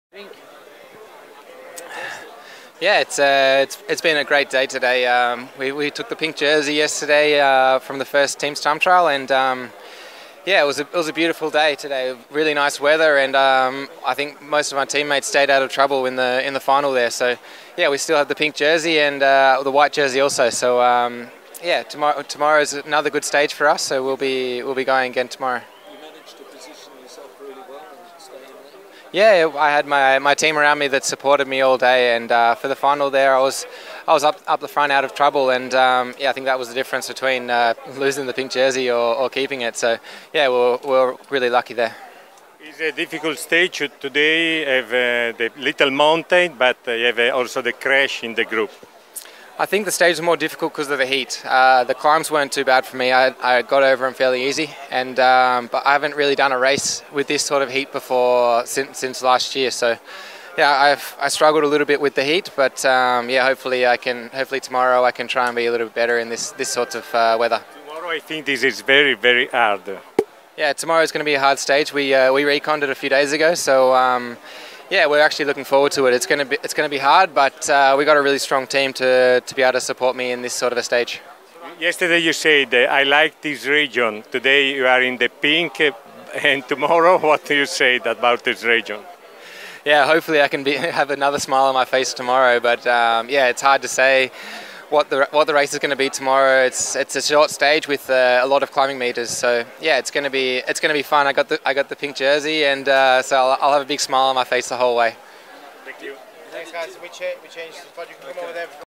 GIro d'Italia 2015 Michael Mattews in Pink Shirt at the end of stage 2
L'australiano Mattews indossa la Maglia Rosa al termine della seconda tappa da Albenga a Genova.